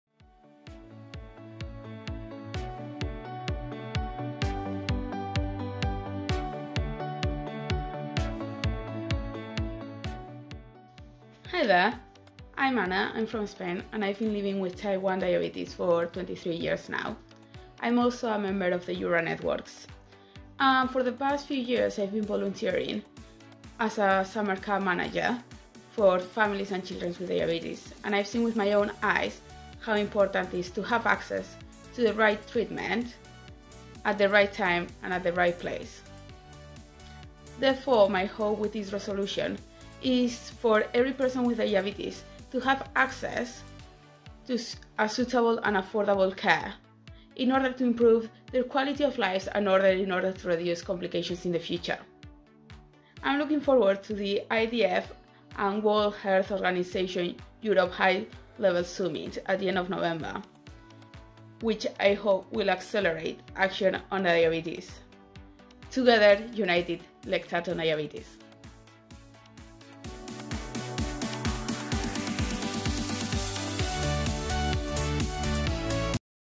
Ana Prats' message on the anniversary of the EU Parliament Resolution on Diabetes
On the occasion of the first anniversary of the European Parliament Resolution on Diabetes, people living with diabetes share their messages to urge the Commission and EU governments to heed  the calls made in the Resolution.

In her message, Ana Prats, a diabetes advocate from Spain, calls for improved access to the right treatment, at the right time and place, for all people living with diabetes to improve their quality of life.

Learn about the importance of uninterrupted, equitable and affordable access to medicines, technologies and care